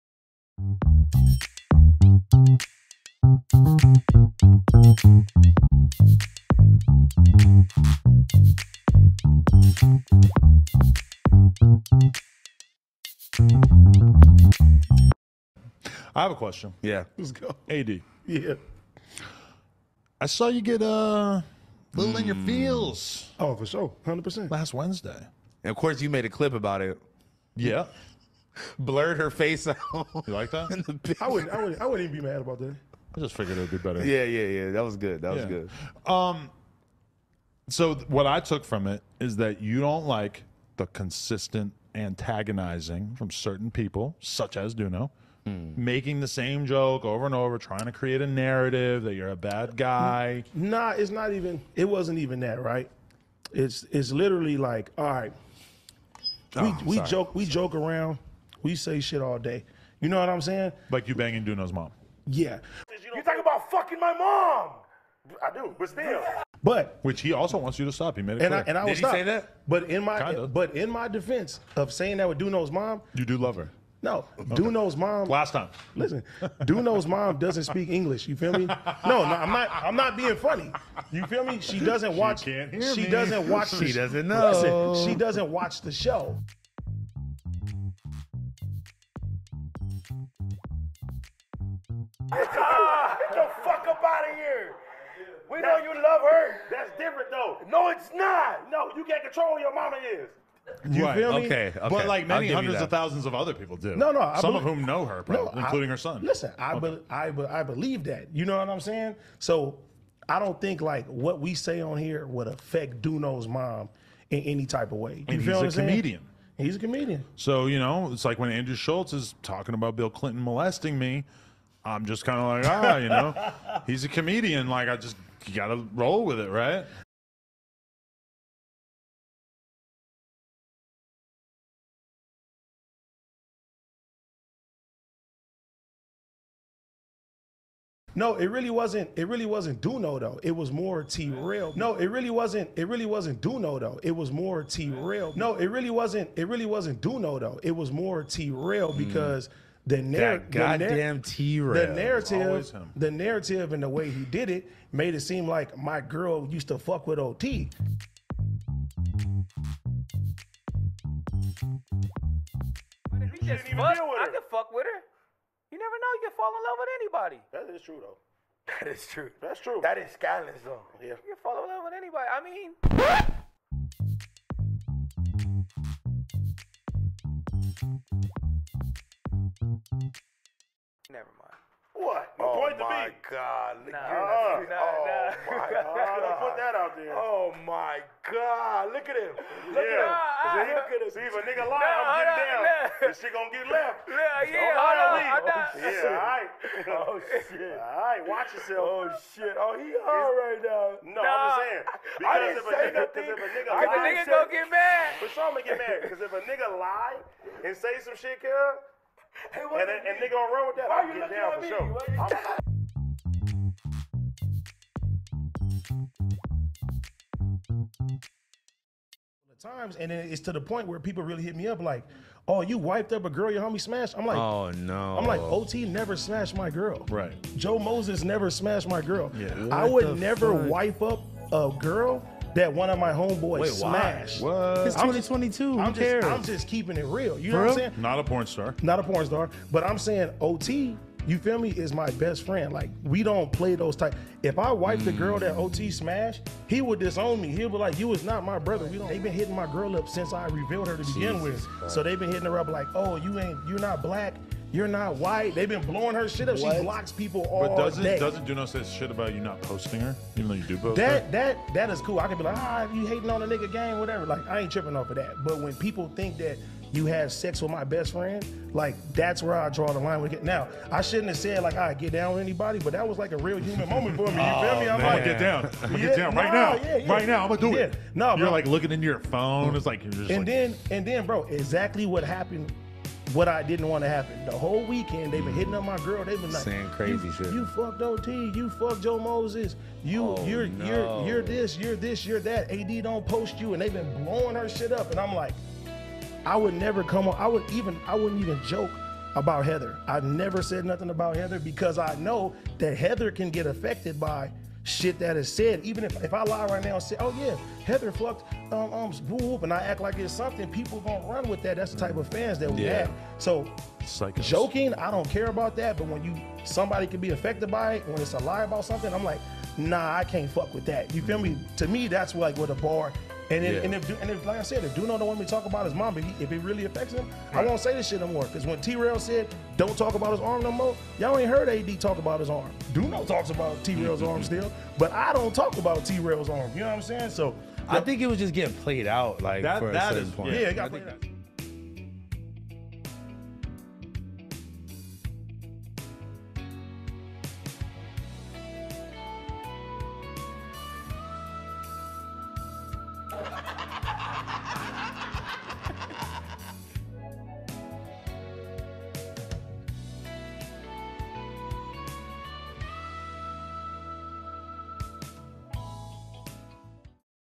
AD Still Sensitive and Upset: **My Best Friend OT Genasis Did Not Smash My Girl**
AD allowed Adam to retrigger him regarding jokes about his relationship.